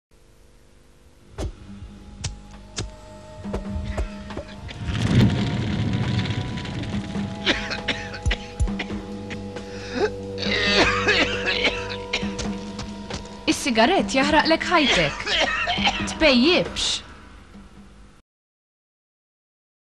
01 D.O.I. Smoking campaign - Is-sigarett jahraqlek hajtek
Produced by RGB Television Productions Malta for D.O.I. in 1992.

Design / Animation/ SFX : Andrew Scicluna
Coughing background voice: John Scicluna